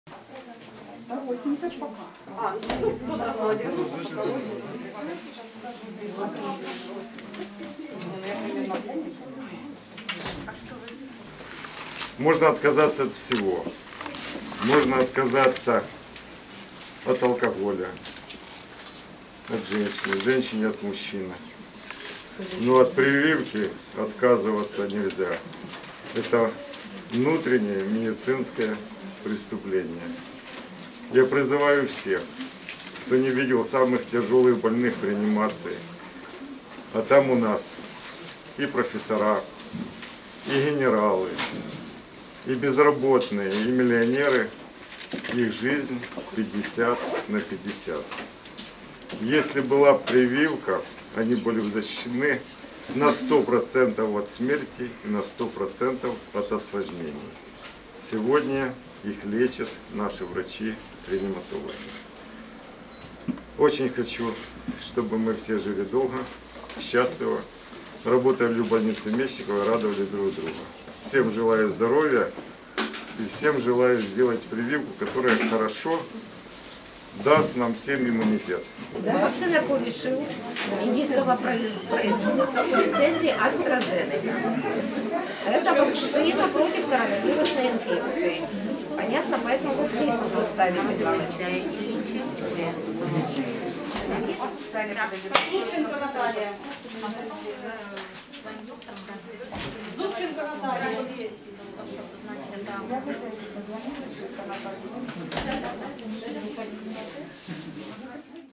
Повторна вакцинація лікарів у лікарні Мечникова
Сьогодні, 19 березня, в Дніпропетровській обласній лікарні імені Мечникова провели чергову вакцинацію проти коронавірусу серед медичних працівників. Індійську вакцину «CoviShield» вкололи генеральному директору Сергію Риженку, ректору Дніпровського медичного університету Тетяні Перцевий та іншим медикам.
Генеральний директор лікарні зазначив, що у лікарів зараз єдина ідеологія. Всі вони розуміють важливість щеплення: вона дасть змогу спокійно працювати і повертатися додому після без страху заразити близьких.
Перед вакцинацією у лікарів заміряли температуру і рівень серцевого тиску. Також уточнювали про наявність у вакцинованих хронічних захворювань і переконувалися у відсутності будь-яких ознак респіраторних захворювань. Загалом сьогодні прищепили 100 медиків, водночас використовували лише 10 ампул індійської вакцини «CoviShield».